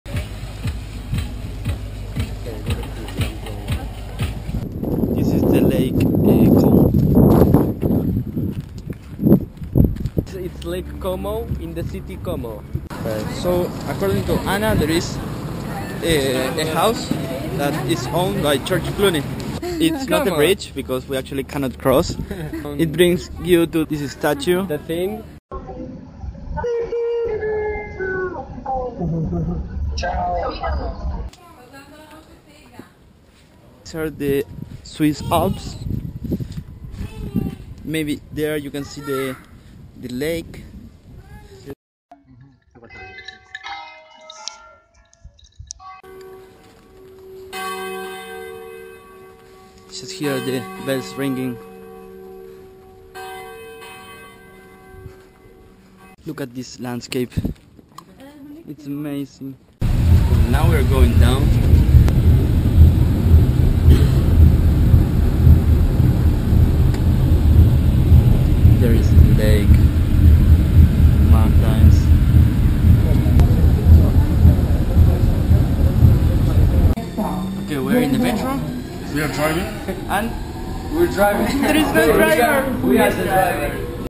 Los Alpes desde el lago Como
Excelente visita fugaz al lago Como, guiados por Anna, que le dio cama a uno de los chicos por couchsurfing y se tomo el tiempo de llevarnos a recorrer el lago.